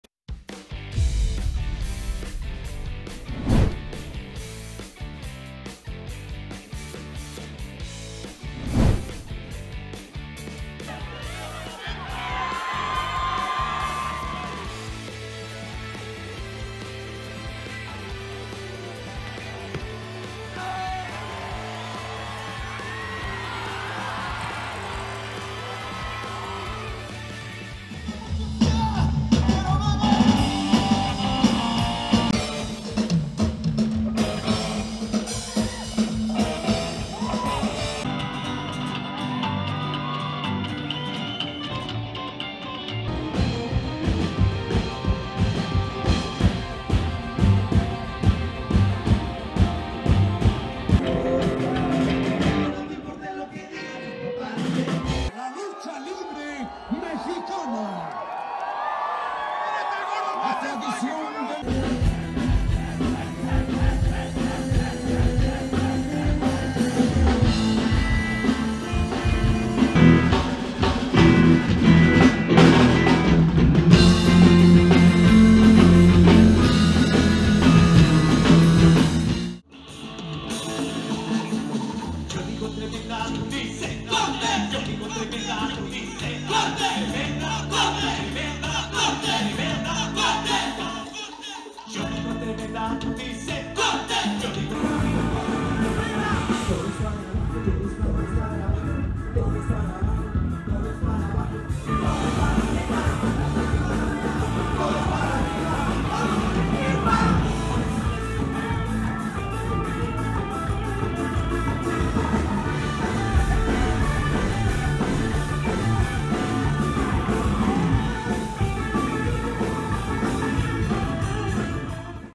FESTIVAL JOVENES POR LA PAZ VOL.1 - MONUMENTO A LA REVOLUCIÓN
🎉Festival Jóvenes Por La Paz🎉 Vol. 1✨

🤼‍♂️¡La lucha libre + música = a combinación perfecta!❤️‍🔥

 Así se vivió el Vol. 1 desde el 📍Monumento a la Revolución☄️, sumergiéndonos en las olas de la música y una función de lucha libre…🌊👀 Agradecemos a las personas que nos acompañaron, ¡nos vemos pronto con más aventuras como esta!🔥✌🏻✨‼️